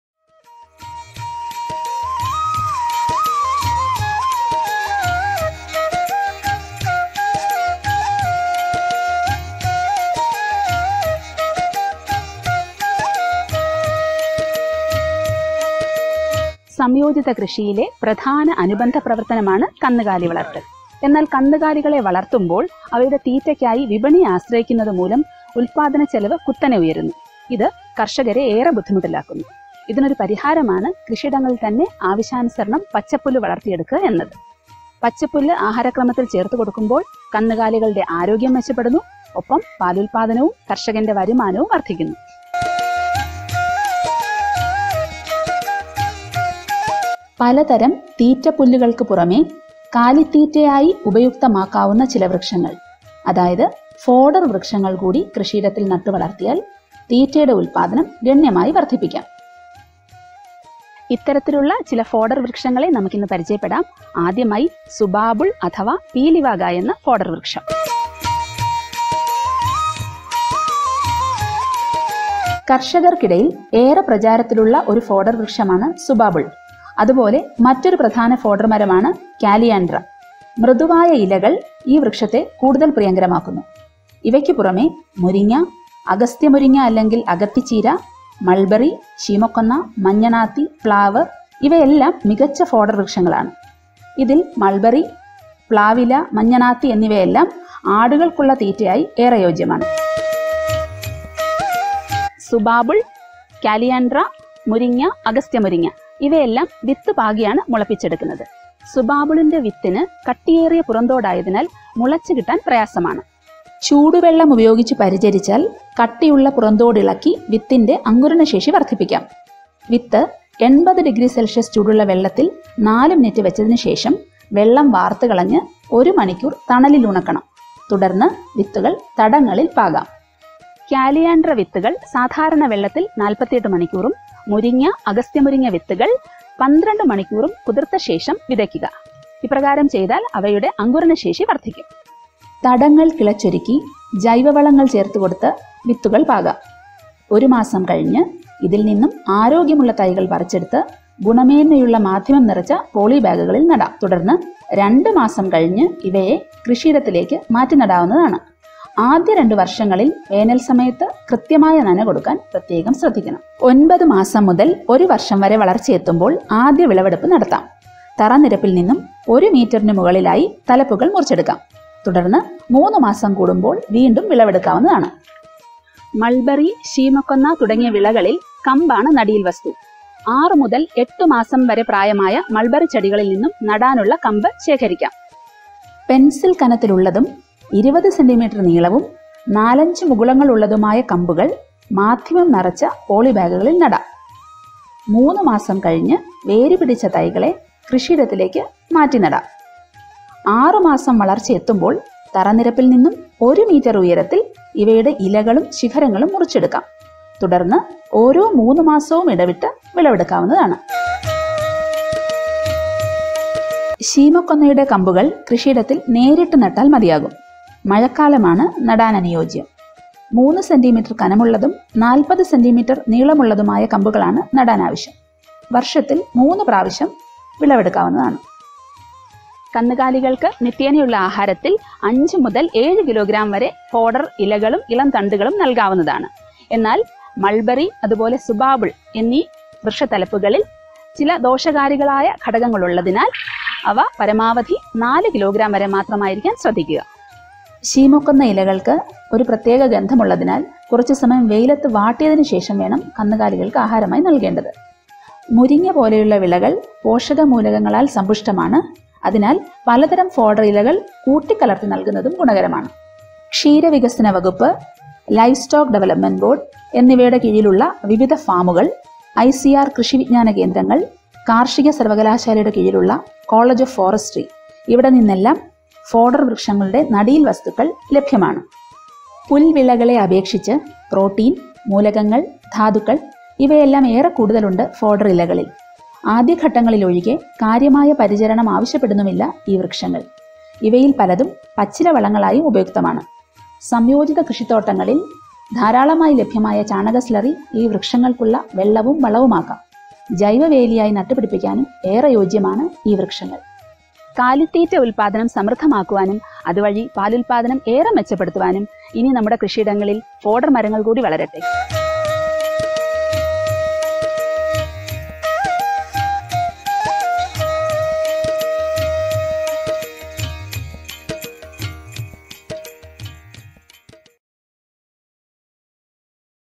IFSRS, Karamana - Krishi Arivukal XV "Tree fodders: The feed reserves in IFS"
Dairying is the most common allied enterprise in IFS. If cattle feeds from markets are relied on too much, the cost of production escalates and dairying becomes uneconomic. This necessitates generation of green fodder within farms. In addition to different fodder grasses, tree fodders also can be raised in crop fields as they serve as perennial feed sources catering to year round requirement. They have nutritious foliage rich in proteins and minerals, and can be grown as biofences as well. Subabul, Calliandra, Moringa, Sesbania, Mulberry, Glyricidia,Jack, Morinda etc are some common fodder trees suitable for Kerala. Stall wash and bio-gas slurry available in integrated farms can serve as nutrient sources for these trees.